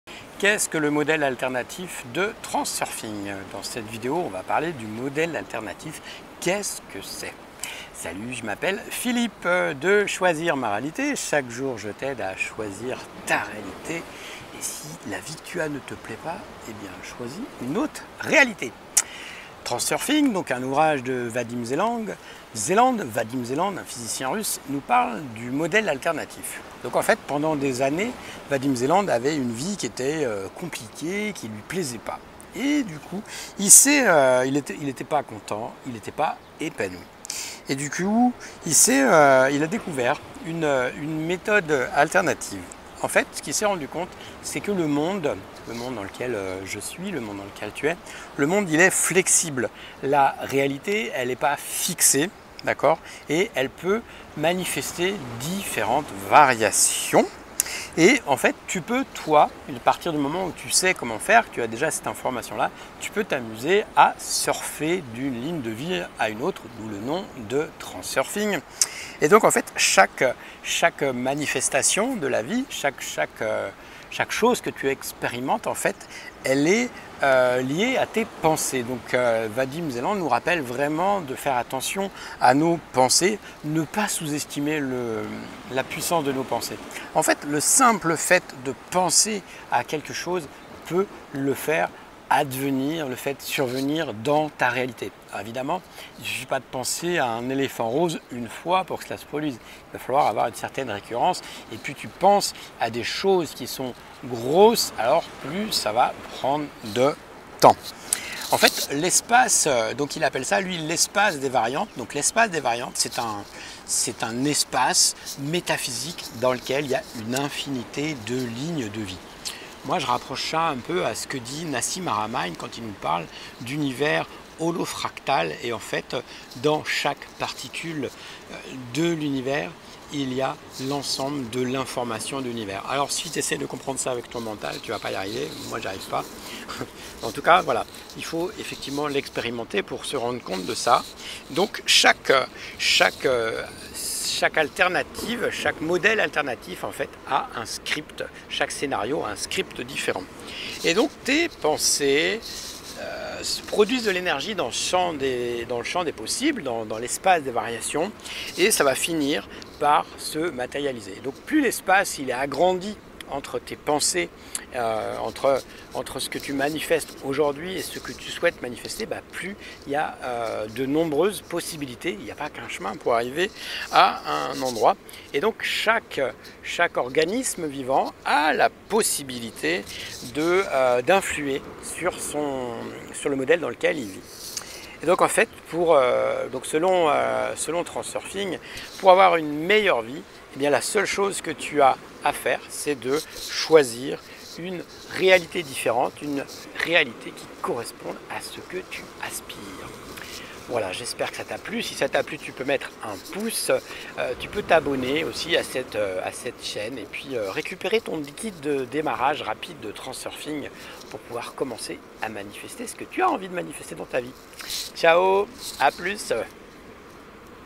TRANSURFING le Modèle alternatif
Qu'est-ce que la Vague du Succès dans le modèle Transurfing de Vadim Zeland ?

📚 Livres Transurfing